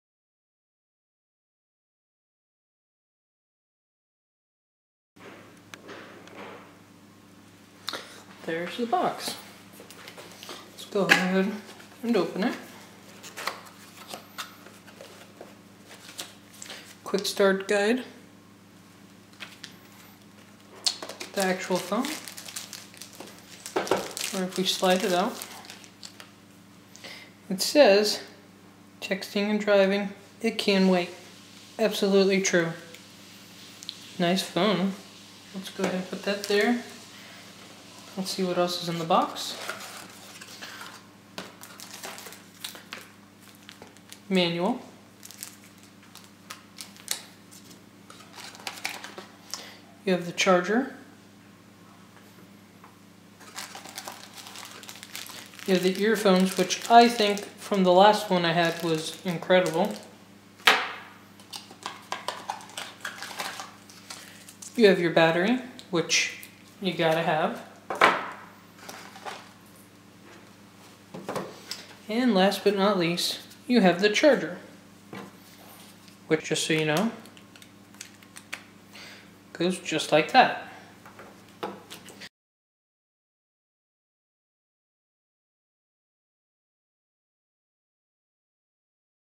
SAMSUNG Focus Flash the Windows Phone 7 - Unboxing
Here on TechMe Show we will be unboxing a brand new Samsung Focus Flash that contains the Microsoft Windows Phone 7 OS and given by TheSocialGeek.com.